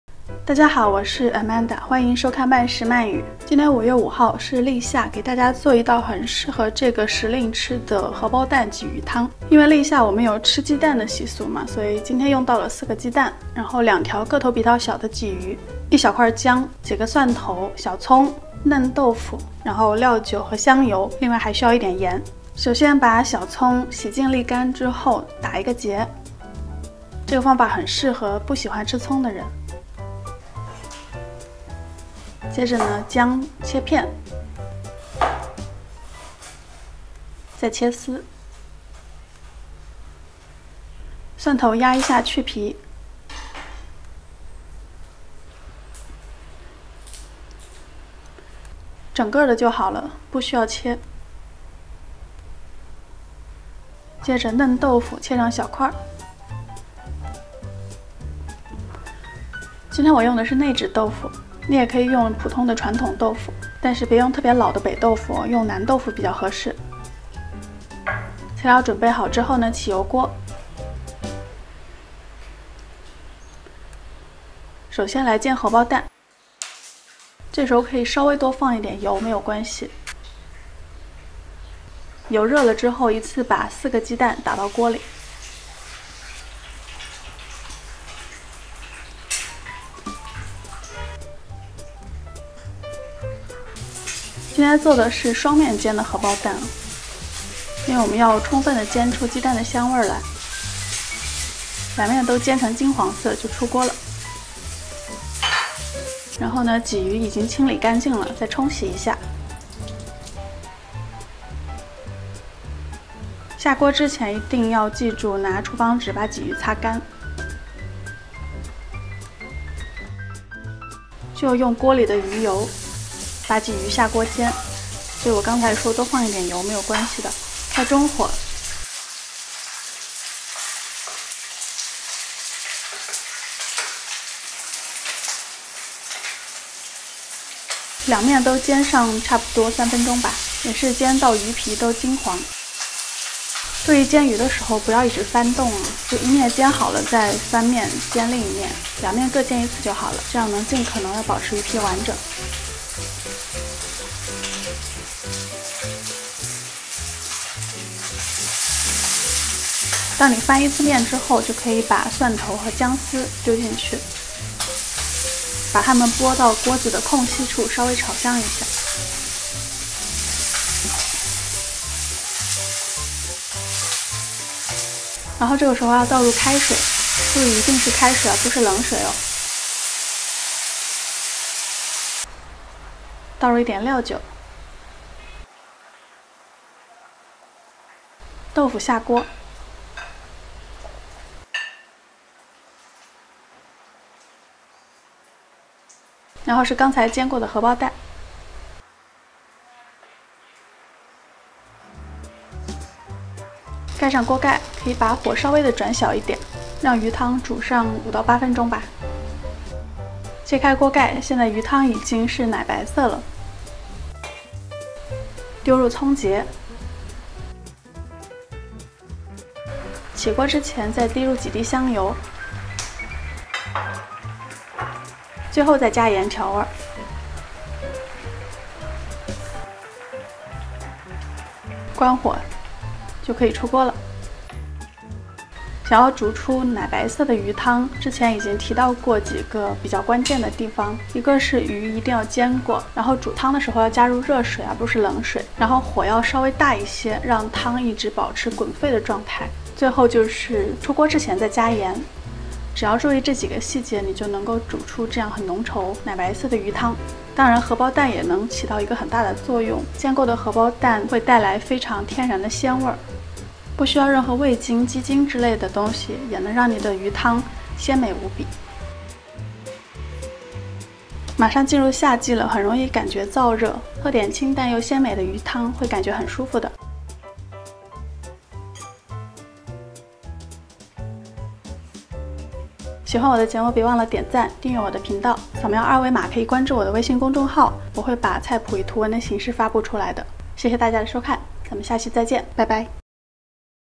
[Eng Sub]荷包蛋鲫鱼汤【曼食慢语】第二季第3集 Fried Egg and Crucian Carp Soup
立夏了，天气一天天燥热起来，喝点清淡鲜美的鲫鱼汤吧~两面煎得金黄的荷包蛋，能给鲫鱼汤增添天然的鲜味，不需要任何鸡精味精之类的添加，鱼汤也能浓郁鲜美。

新浪微博：@Amanda的小厨房
微信公众号：amandatastes
片尾有二维码，扫一扫即可关注微信哦